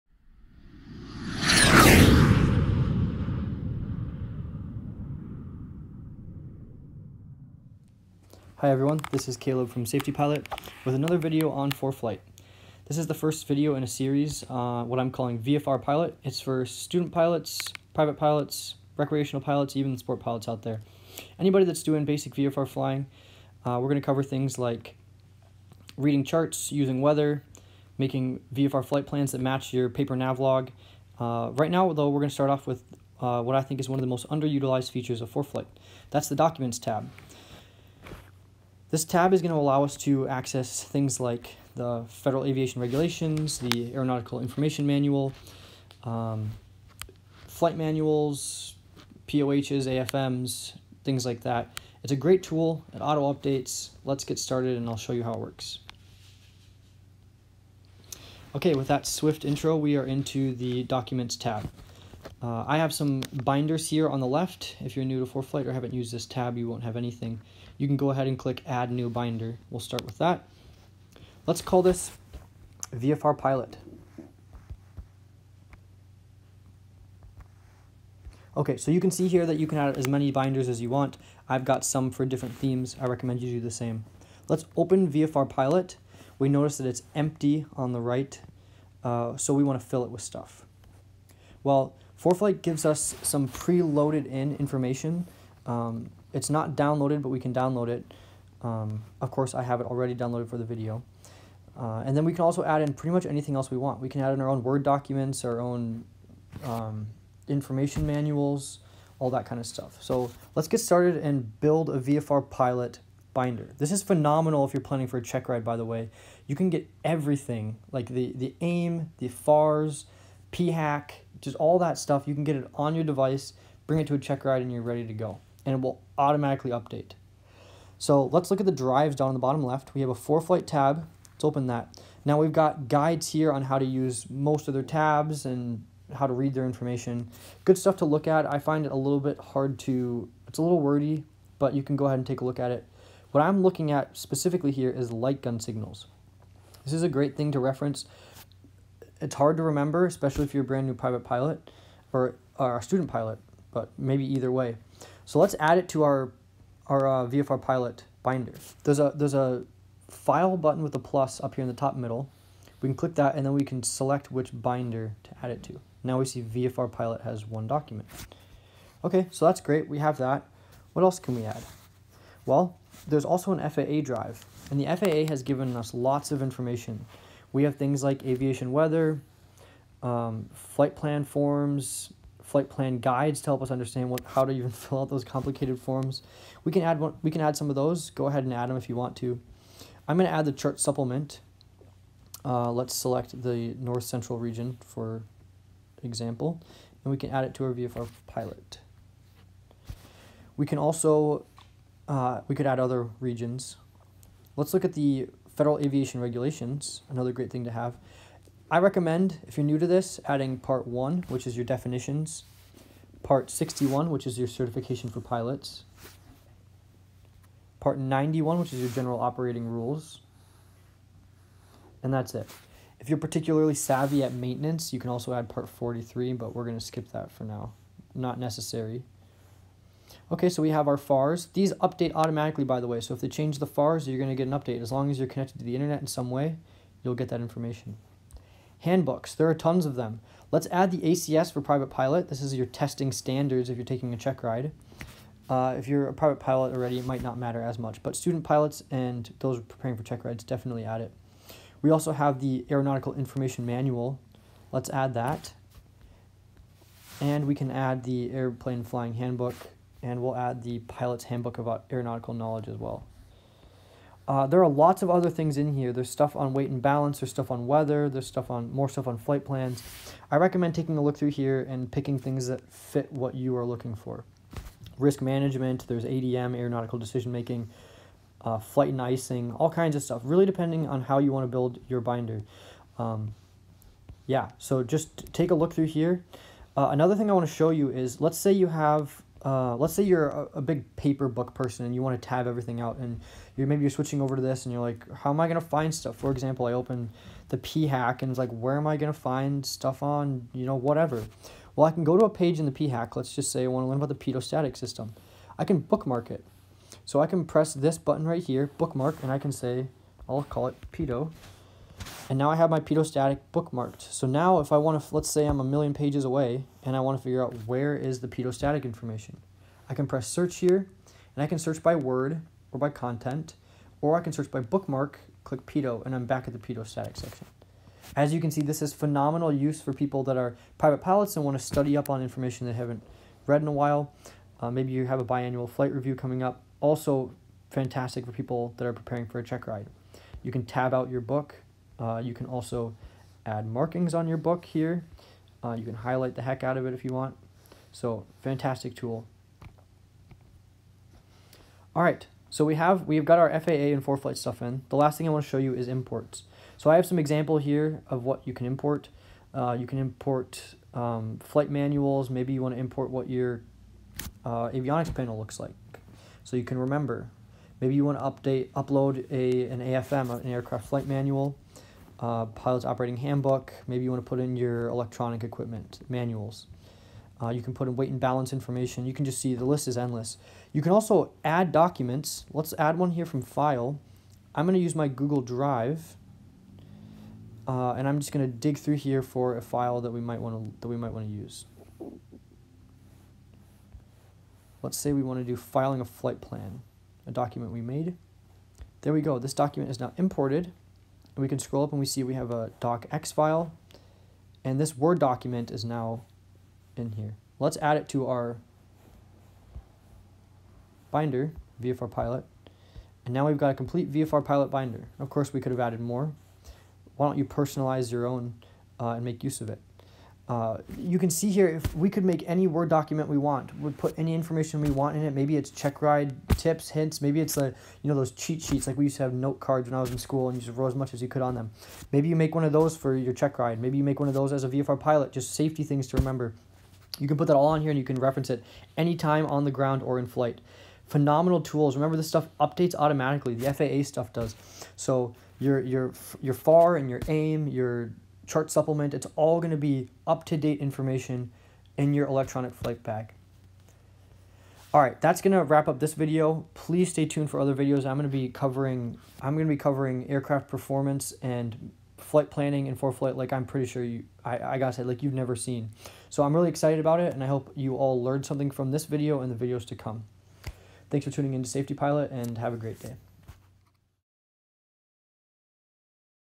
Use Foreflight and Never Pay for Another Flight Training Book Again
Want to carry your airplane's POH with you, know exactly where to find VFR Sectional Legends, and never have to buy a FARAIM again?!

In the first video of the VFR Pilot Foreflight series, we explore perhaps the most underappreciated function on Foreflight, the Documents Tab. Learn to assemble a comprehensive set of resources to enhance your learning and change the way you find information.

Seriously, Foreflight lets you keep all the study materials you will ever need all in one place, and has convenient features for bookmarking and editing. It will change the way you prepare for FAA exams and make you perform better.

Check back for more VFR Pilot Foreflight videos